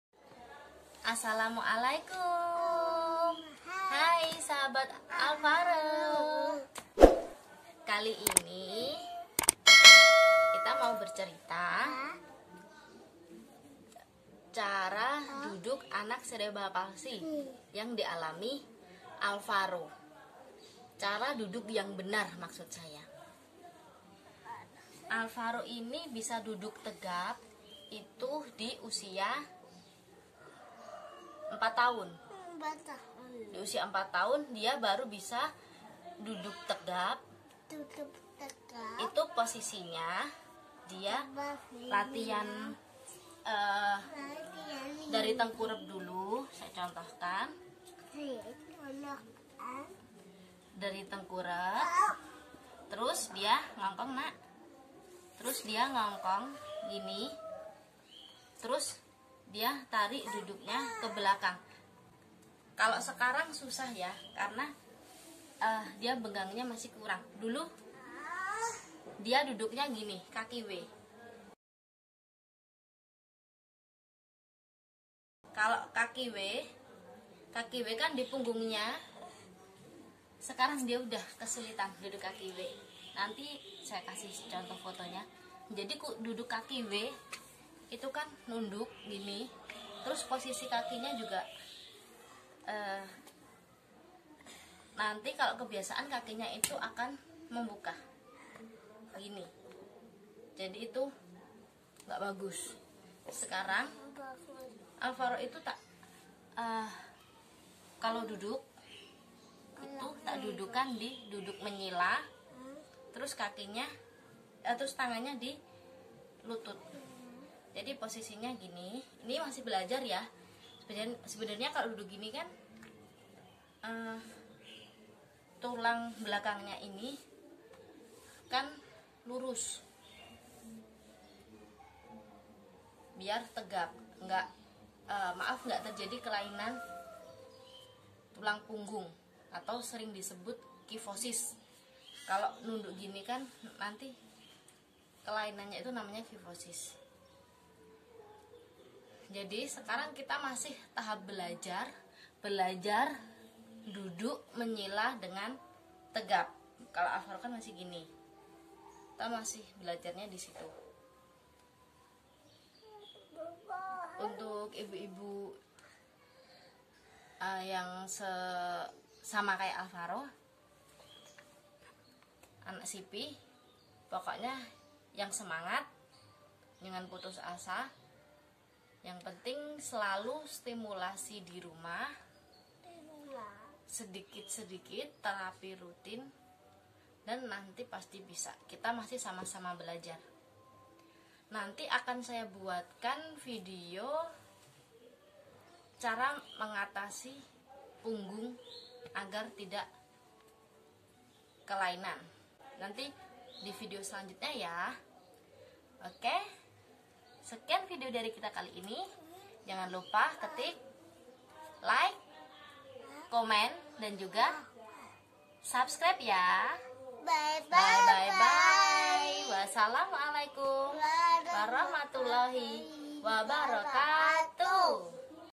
cara duduk yang benar anak cerebral palsy , how to sit properly
assalamu'alaikum

kali ini mamah alvaro mau bercerita soal duduk yang benar . teruma untuk alvaro

duduk yang benar untuk anak cerebral palsy itu punggung tegap , kepala tegap . kalau posisi duduk dibawah lebih baik menyila
tapi ya sebaiknya kalau duduk jangan lama lama ya

alvaro bisa dudu tegap usia 4 tahun teman teman . di usia itu duduk nya pun kaki masih W  . kalau keterusan duduk kaya gitu tidak bagus untuk perkembangan kaki nya . bisa menyebabkan kaki , maaf W . setelah bisa duduk tegap tadi dengan posisi kaki W . kita mulai mengajarkan alvaro untuk duduk dengan sila
pertama kali di ajarkan duduk sila , alvaro itu jatuj kebelakang . dan itu menyebabkan untuk anak" . karena kepala bagian belakang itu kan lebih rawan ya ...

saya mengajarinya , saya duduk kan alvaro di tembok posisi sila . trus usahakan punggung nya itu lurus. alhamdulillah duduk sila juga sudah bisa . tapi kadang badan masih condong ke depan . dan itu kurang baik untuk pertumbuhan tulang belakang nya . alvaro masih semangat belajar ya .
untuk teman teman yang special kaya alvaro , tetap semangat ya . kita semua pas
ti bisa . banyak orang orang yang menyayangi kita ,

semangatt ya